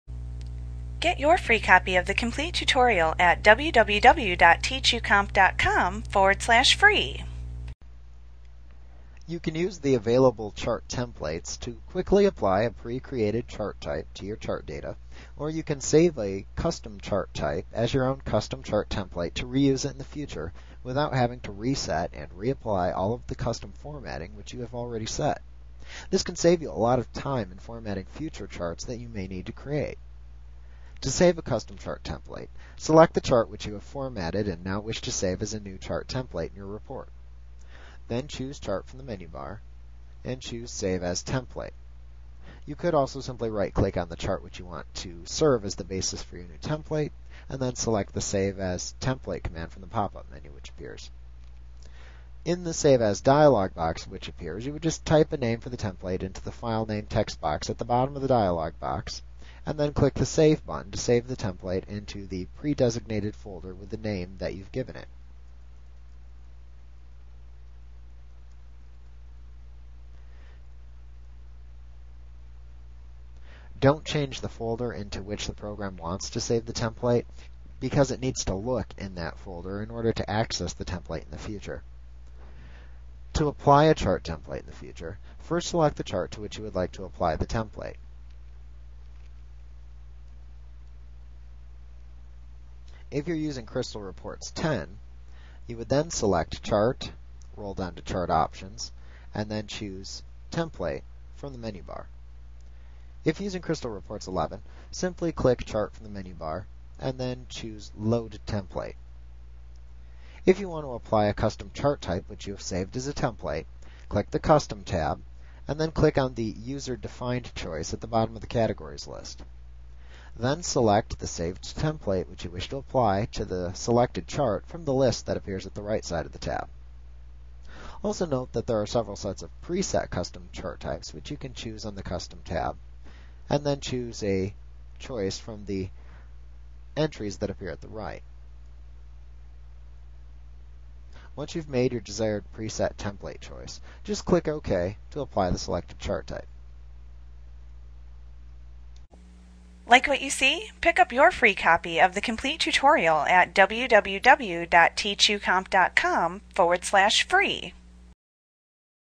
Crystal Reports Tutorial Using Chart Templates Business Objects Training Lesson 12.14
Learn how to use chart templates in Crystal Reports by Business Objects A clip from Mastering Crystal Reports Made Easy v. 11. - the most comprehensive Crystal Reports tutorial available. Visit us today!